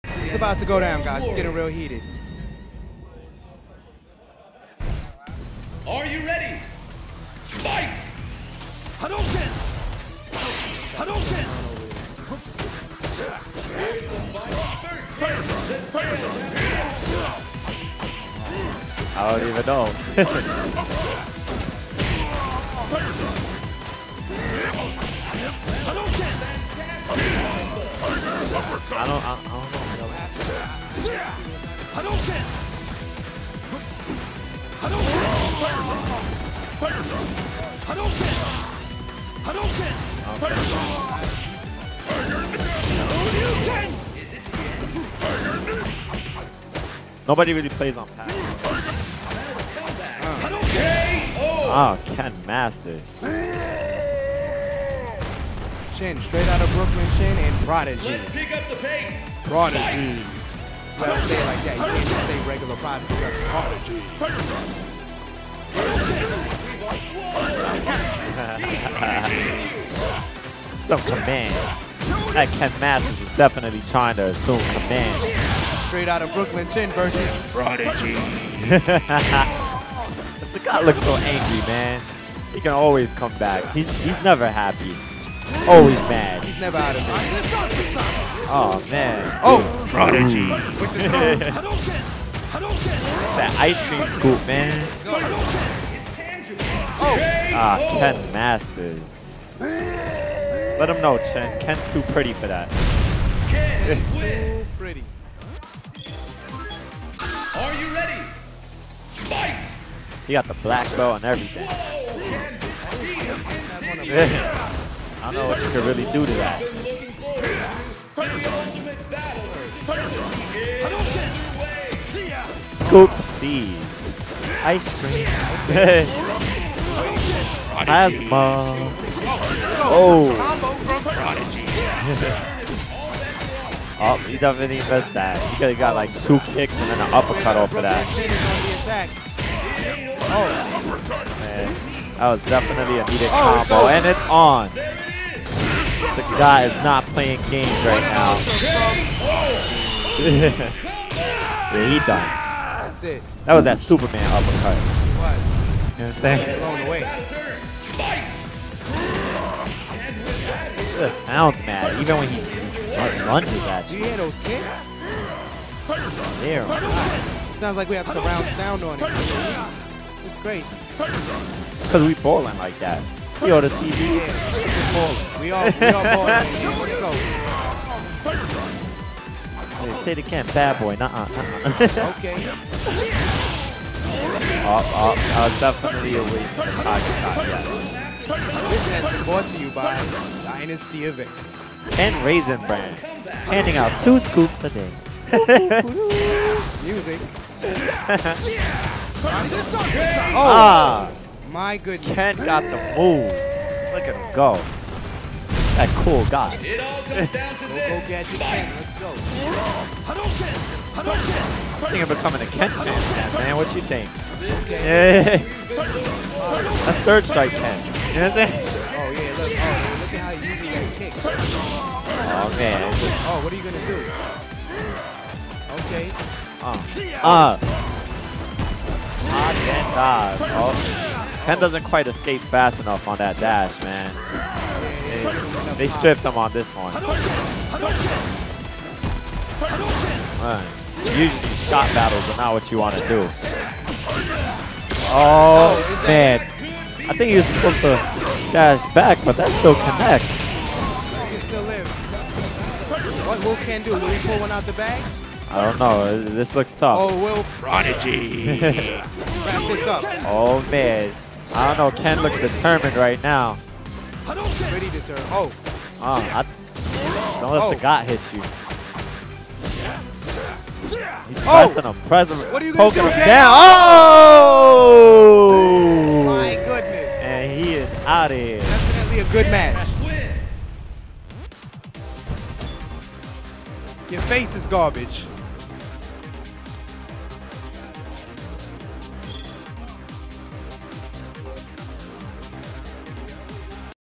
Chin (KEN) vs Prodigy (SAG) Dynasty Events #2
Chin (KEN) vs Prodigy (SAG) Dynasty Events #2 Losers Bracket Match